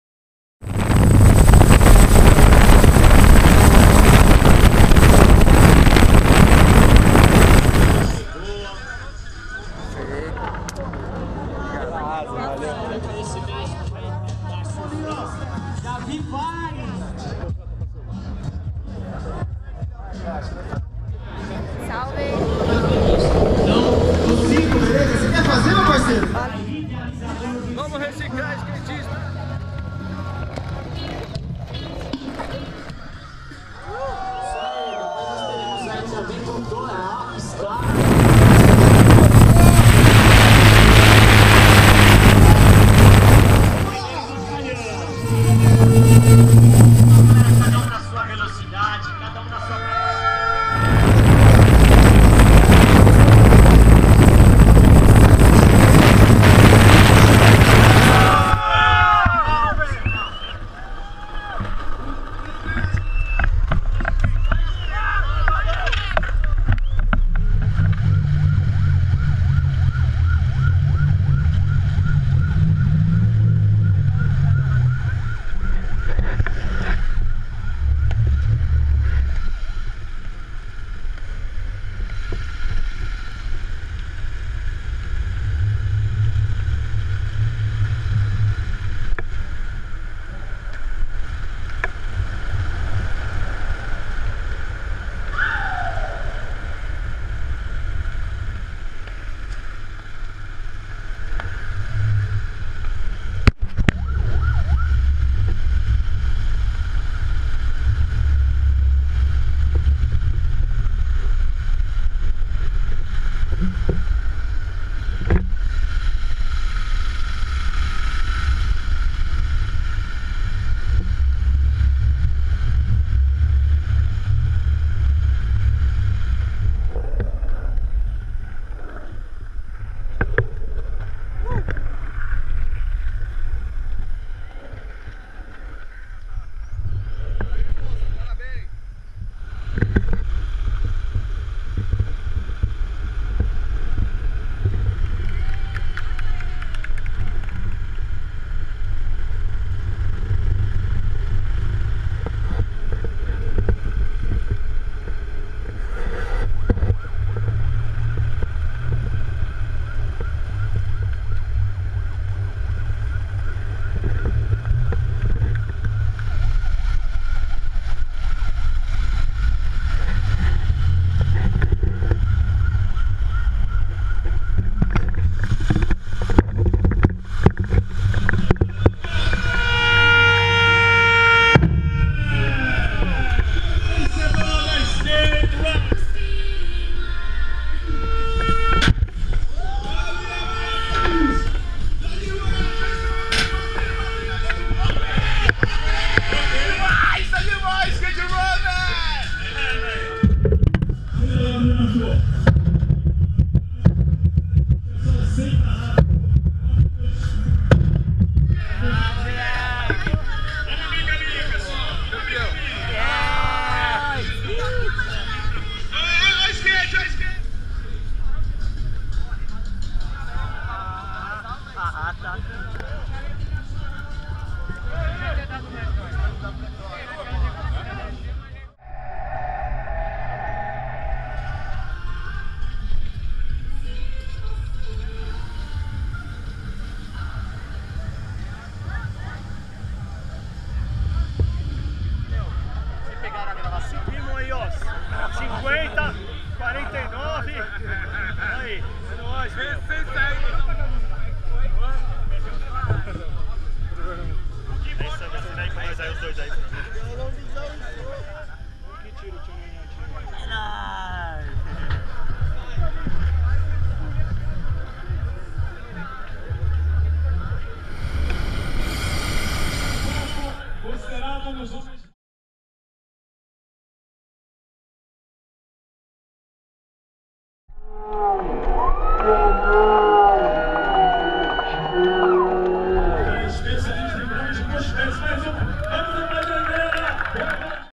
Skate Run 2013 - Melhores Momentos
Confira os melhores momentos da primeira edição da Skate Run, que aconteceu em São Paulo, no dia 22 de setembro de 2013, durante a Virada Esportiva.